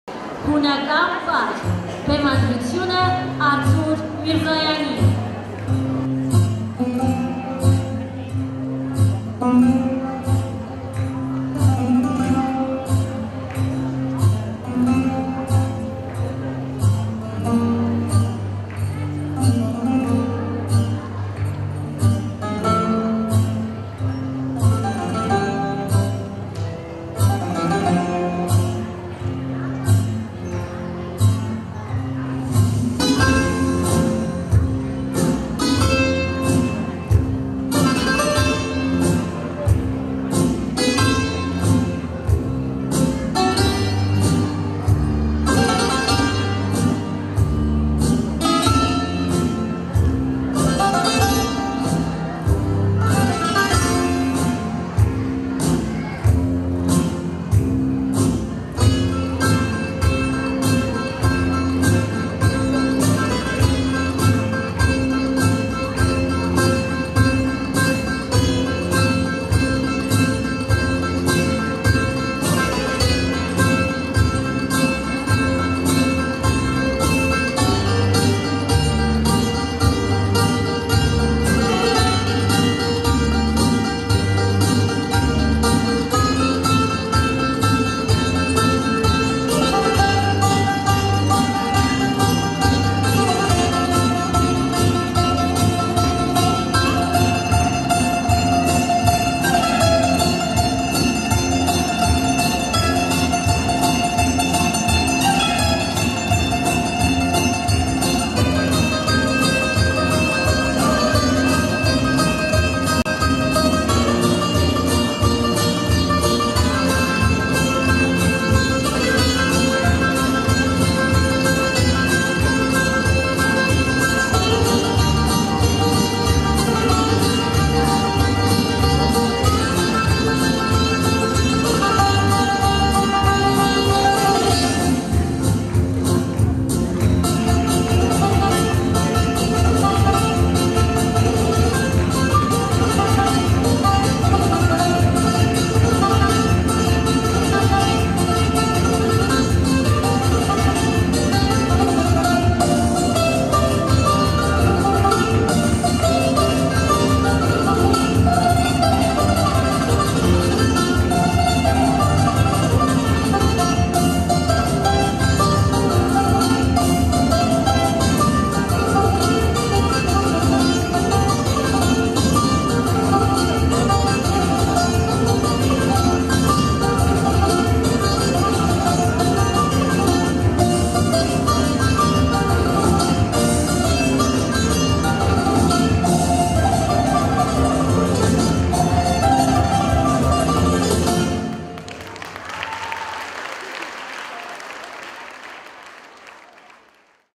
՛՛ԱՆԻ՛՛պարային ստուդիա-Հունական պար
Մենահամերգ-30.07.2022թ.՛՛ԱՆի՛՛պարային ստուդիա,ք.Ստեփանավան